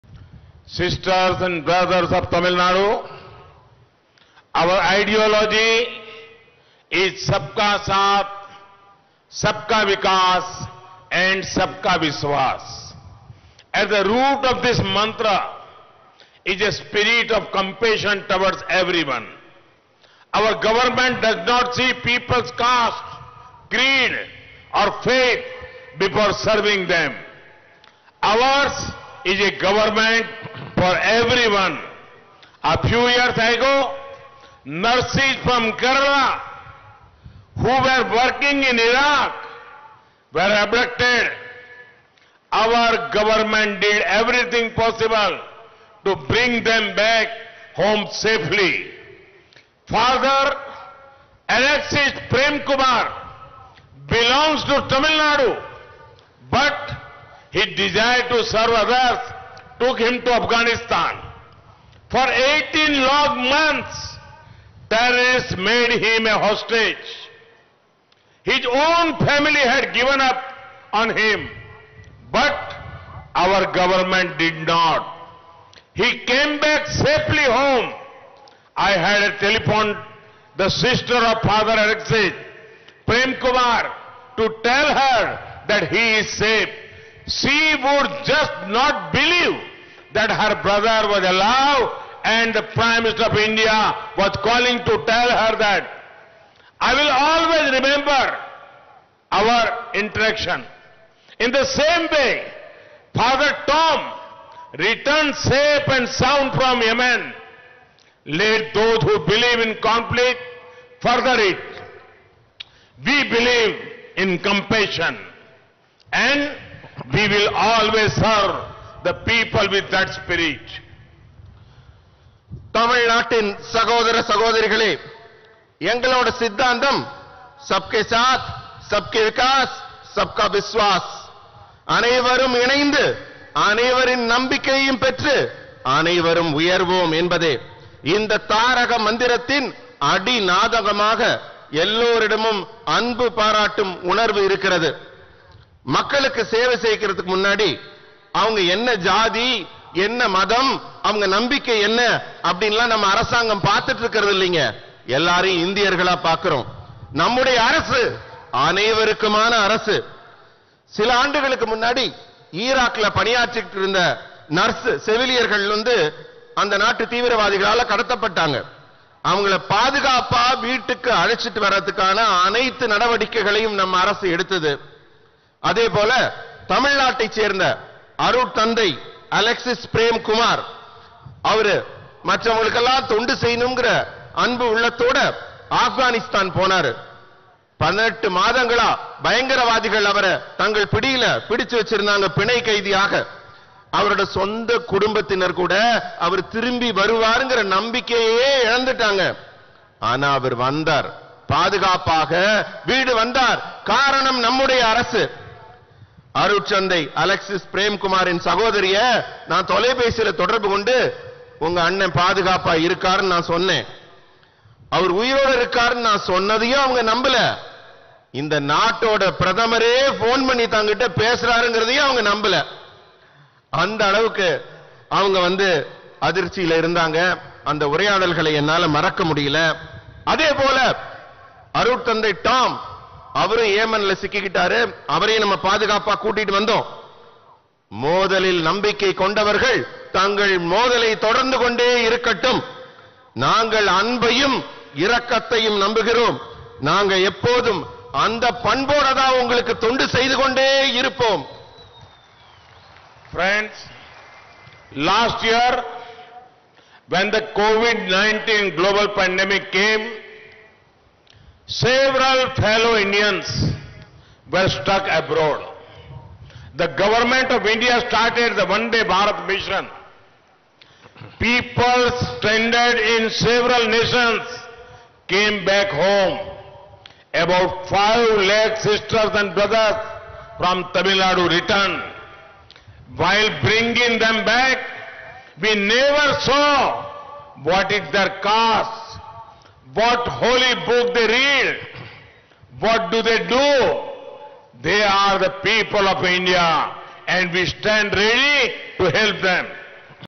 Our ideology is Sabka Saath, Sabka Vikas and Sabka Vishwas: PM
PM Modi addressed election rallies in Tamil Nadu's Madurai and Kanyakumari. He invoked MGR's legacy, saying who can forget the film 'Madurai Veeran'. Hitting out at Congress, which is contesting the Tamil Nadu election 2021 in alliance with DMK, PM Modi said, “In 1980 Congress dismissed MGR’s democratically elected government, following which elections were called and MGR won from the Madurai West seat. The people of Madurai stood behind him like a rock.”

Prime Minister Narendra Modi addresses public meeting at Kanyakumari, Tamil Nadu